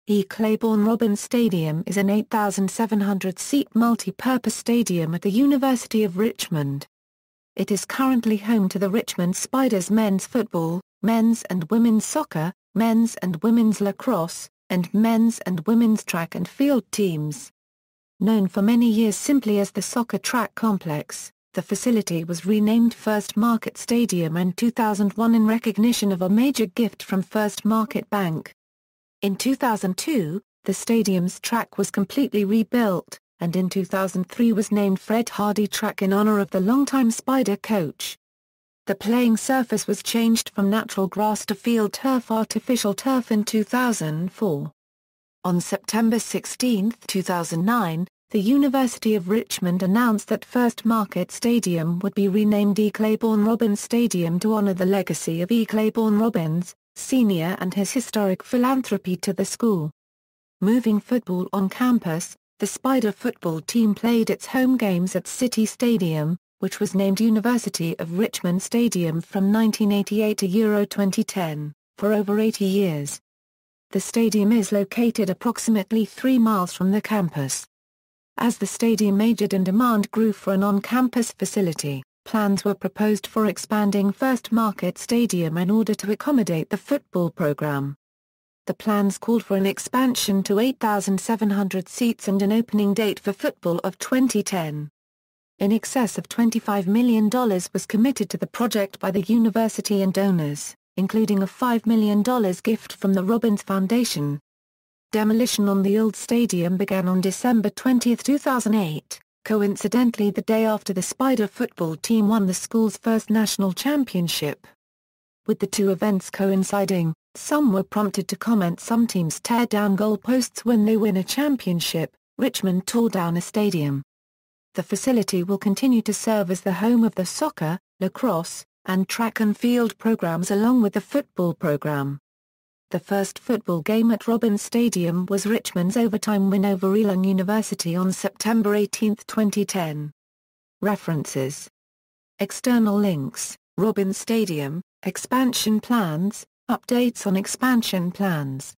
E. Claiborne Robins Stadium
E. Claiborne Robins Stadium is an 8,700-seat multi-purpose stadium at the University of Richmond. It is currently home to the Richmond Spiders men's football, men's and women's soccer, men's and women's lacrosse, and men's and women's track and field teams. Known for many years simply as the Soccer/Track Complex, the facility was renamed First Market Stadium in 2001 in recognition of a major gift from First Market Bank. In 2002, the stadium's track was completely rebuilt, and in 2003 was named Fred Hardy Track in honor of the longtime Spider coach. The playing surface was changed from natural grass to FieldTurf artificial turf in 2004. On September 16, 2009, the University of Richmond announced that First Market Stadium would be renamed E. Claiborne Robins Stadium to honor the legacy of E. Claiborne Robins, Sr. and his historic philanthropy to the school.